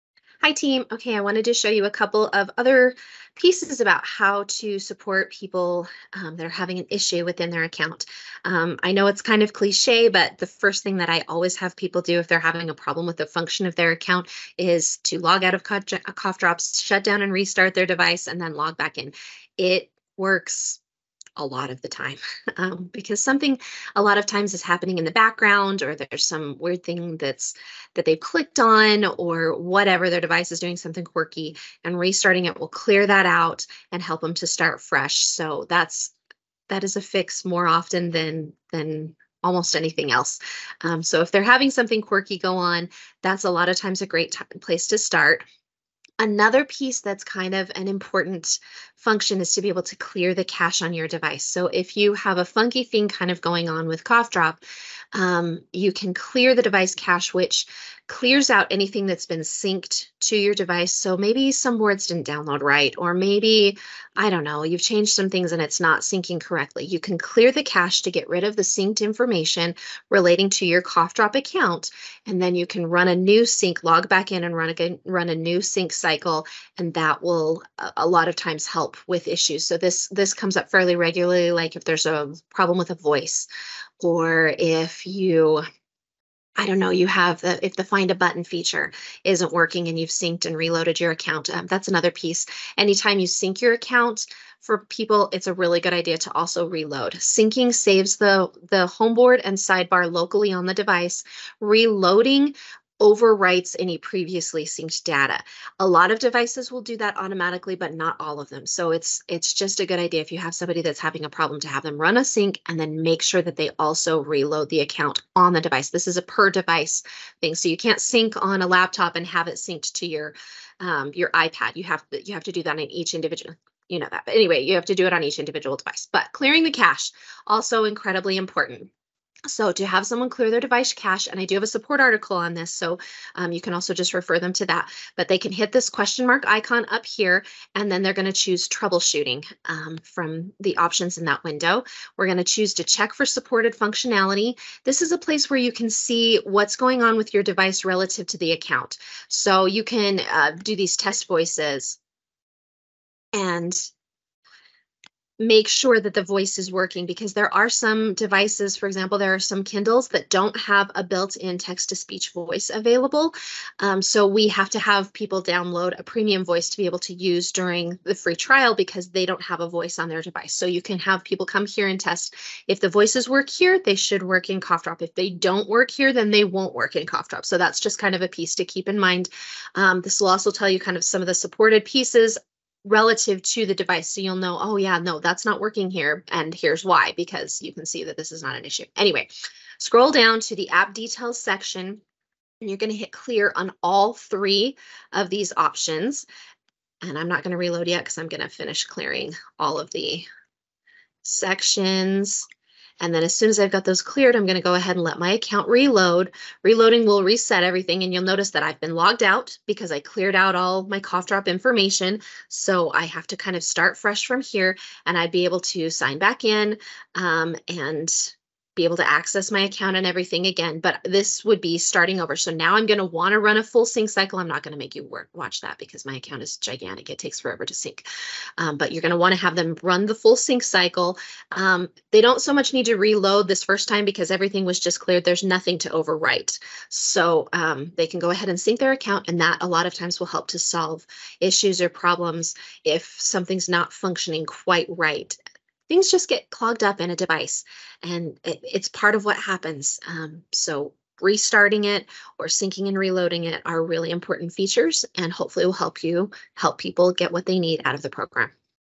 CoughDrop Support for Employees 7: Clearing the device cache
Clear the device cache